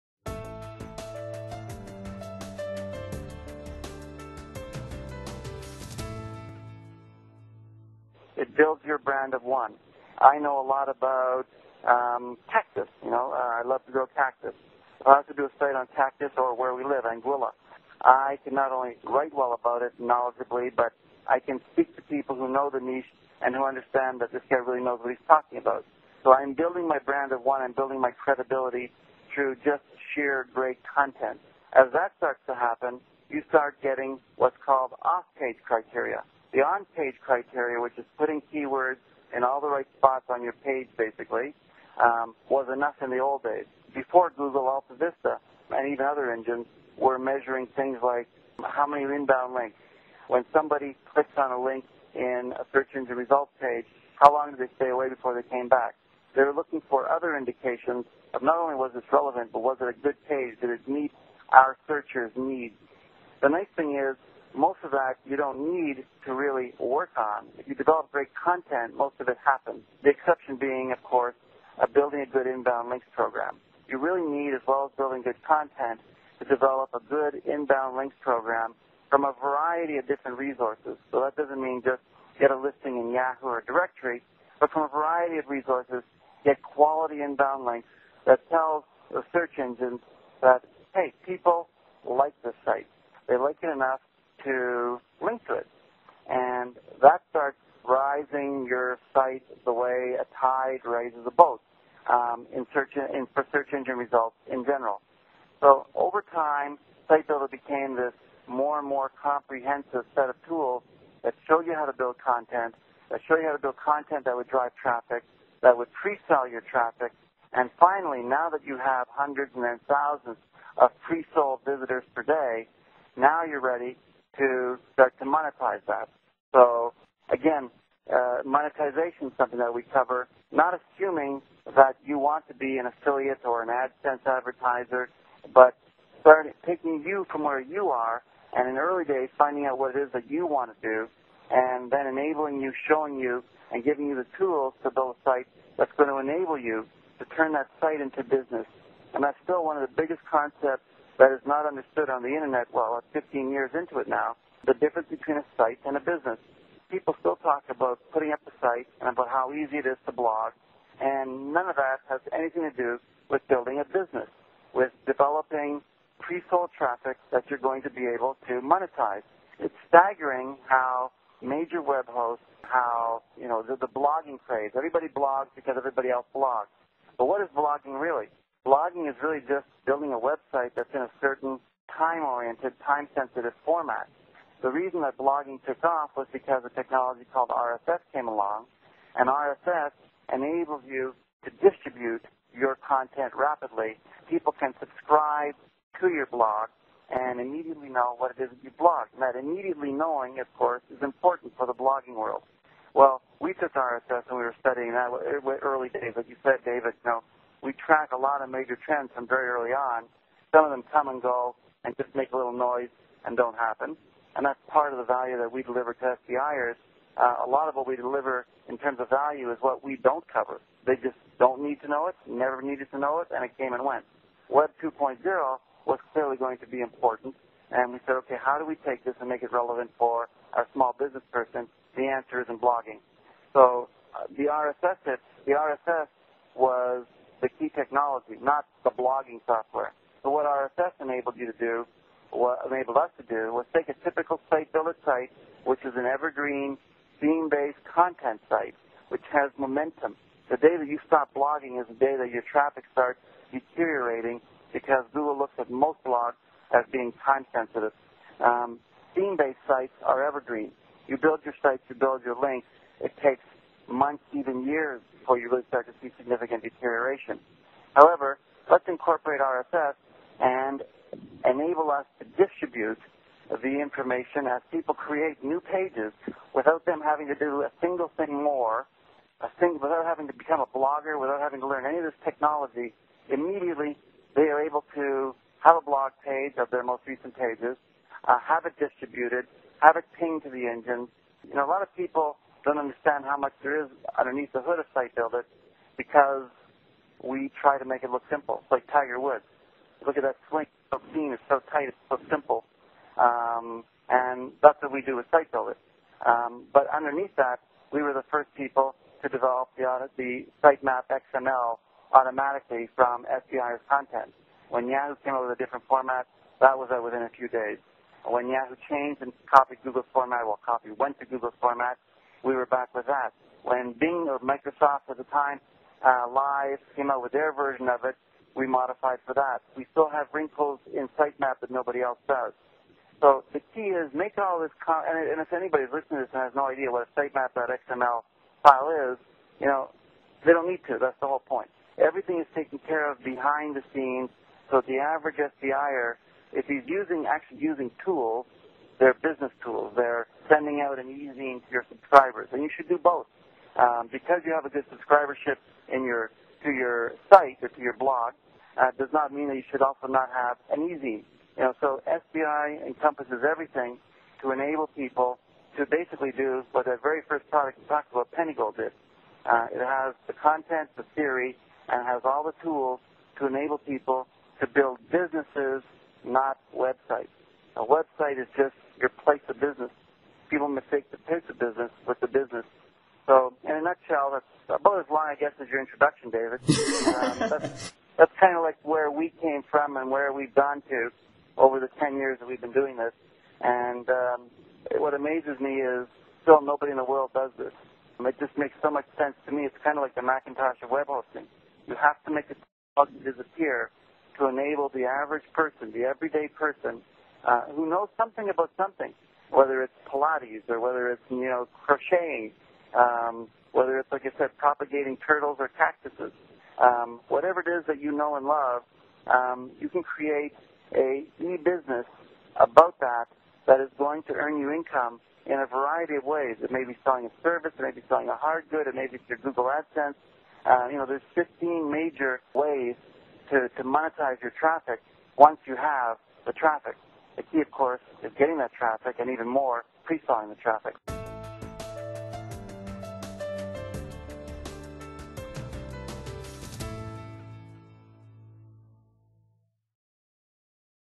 Ken Evoy On Developing Content For Your Site
Ken Evoy details the importance of developing good site content for your website. How to take the first steps, picking a narrow topic you are expert in. How to optimize search engines activity to get the best hits for your site. It's not about keywords only though.
Ken explains how to build your brand on this topic, and where are the most important starting points. There are several onpage and offpage criteria you should consider.
How to search, link, share; which are the best platforms. Find more about the use and drawbacks of business blogs and RSS content. How can sitemaps as business tools help you?